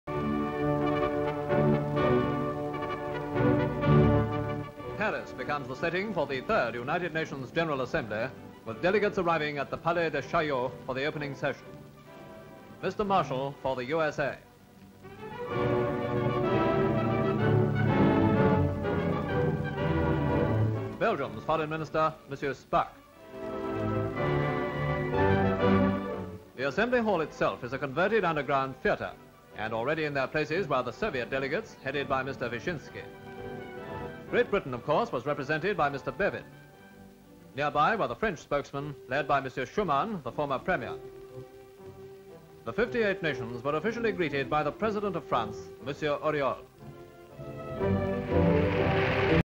Third UN General Assembly meets in Trocadero (1948)
Delegates arriving for the opening session as the United Nations holds the third General Assembly meeting at Palais de Chaillot

Full Description:

SLATE INFORMATION: United Nations Assembly in Paris

FRANCE: Paris: Palais de Chaillot:
EXT/INT

M. Vincent Auriol - President Officially greeting the 58 nations present at the 3rd United Nations General Assembly at Palaid de Chaillot, Paris

BEVIN Represents Gt. Britain at 3rd United Nations General Assembly at Palais de Chaillot, Paris

EVATT, Dr. (Australian Minister in U.S). Elected President of the United Nations Assembly by vote and makes speech on his heavy and responsible task

FLAGS Shots of flags outside the Palais de Chaillot for 3rd United Nations Assembly

Dr. GARBETT. (A-B of YORK.) Right Hon. & most Rev./Cyril Forster. G.C.V.O., D.D. Message to people on the problem s and hopes of peace Good close-up talking into camera

MARSHALL Seen arriving fro 3rd United Nations General Assembly at Palaide Chaillot. Paris

M. ROBERT SCHUMAN - PREMIER OF FRANCE French spokesmen led by M. Schuman at 3rd United Nations General assembly at Palais de Chaillot

SPAAK Representing Belgium at the 3rd United nations General Assembly at Palais de Chaillot in Paris

U.N.O. Paris - setting of third United Nations General Assembly. Delegates arrive at Palaid de Chaillot. Mr Marshall for U.S.A. Mr Vyshinsky, Mr Bevin, Mr Schuman. 58 nations officially greeted by President of France, M. Auriol. At casting of votes for President of Assembly won by Dr Evatt

VISHINSKY M. Heads the Soviet delegation in Third United Nations General Assembly at Palaid de Chaillot, Paris

France

Background: Delegates arriving for the opening session as the United Nations holds the third General Assembly meeting at Palais de Chaillot